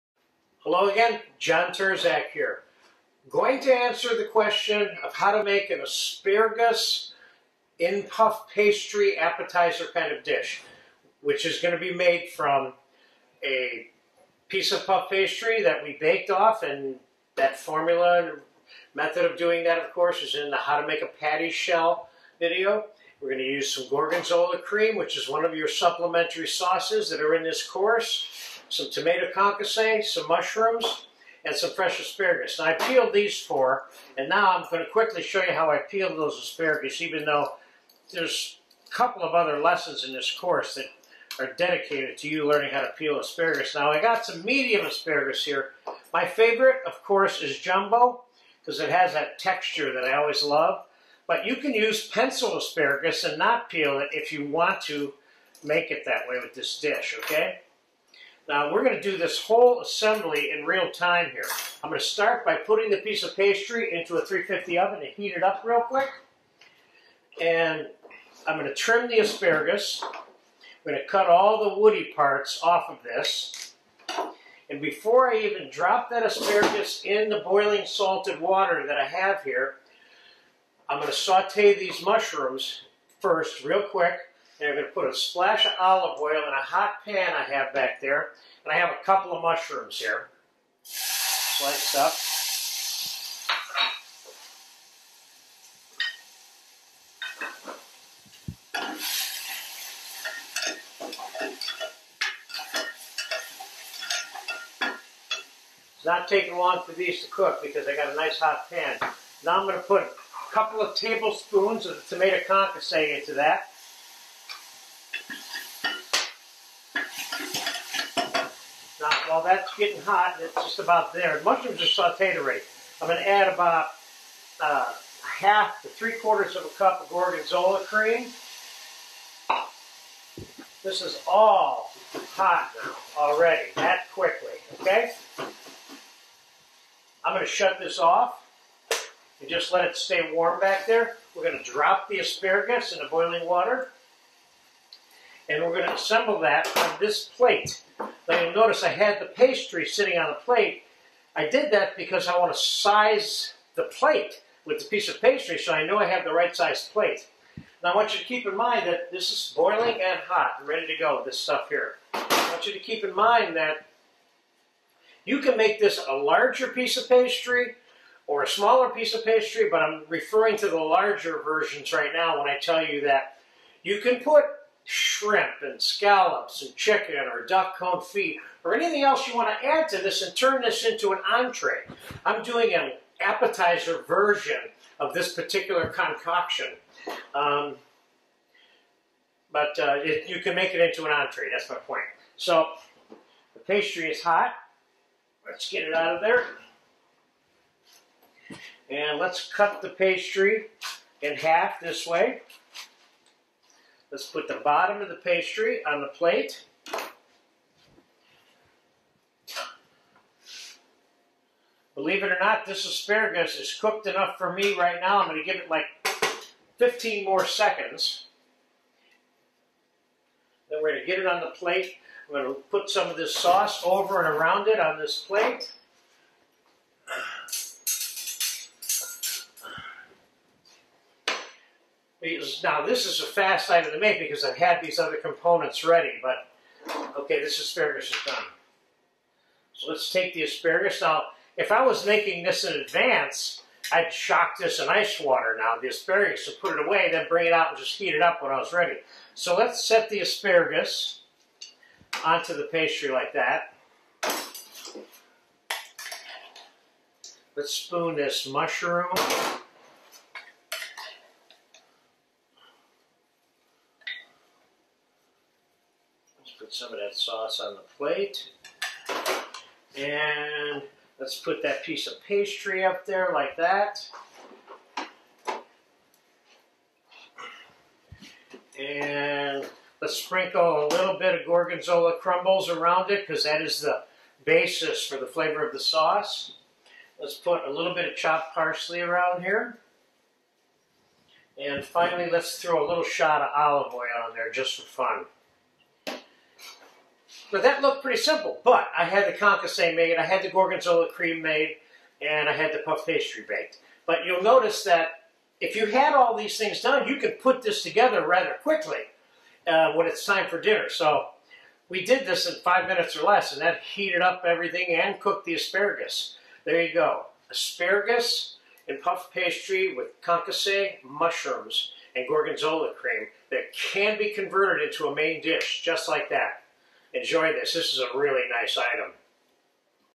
Vintage-2013- Asparagus w Ragout n Puff Pastry - RV KITCHEN
DESCRIPTION
In this video you'll see a rectangle of cooked puff pastry cut in half, then sandwiching a combination of warm asparagus, gorgonzola cream, mushrooms and tomato concasse within it, and then plated as an appetizer.

INGREDIENTS
6 medium to large fresh asparagus - peeled with the "woody ends" cut off
2 tablespoons tomato concasse
1/2 to 3/4 cup gorgonzola cream sauce
2-3 whole white mushrooms - sliced
1 piece baked puff pastry rectangle/patty shell
2-3 tablespoons extra virgin olive oil
1 heaping tablespoon gorgonzola crumbled or any bleu cheese
1 tablespoon fresh chopped parsley

METHOD
Place the pastry in a 350 degree oven to warm up as you prepare to complete the dish.  Yes, this item can be made & assembled in the time it takes to warm up the pastry. Drop the asparagus into boiling water to cook.  This should take 2-3 minutes, depending on the size of your asparagus.  Place 2 tablespoons of the olive oil into a hot saute or small saucepan.  Add the mushrooms and saute for a minute.  Add the tomato concasse and the gorgonzola cream and bring that to a quick simmer and shut it off.  Remove the pastry from the oven and cut it in half lengthwise and place the bottom half onto the serving plate.  Using tongs, transfer the cooked asparagus onto the pastry.  Spoon the sauce/food mixture over the asparagus and onto the plate.  Place the pastry top on top of the asparagus and distribute the gorgonzola crumbles around the pastry.  Drizzle with a little olive oil and sprinkle with chopped parsley.
This item is complete.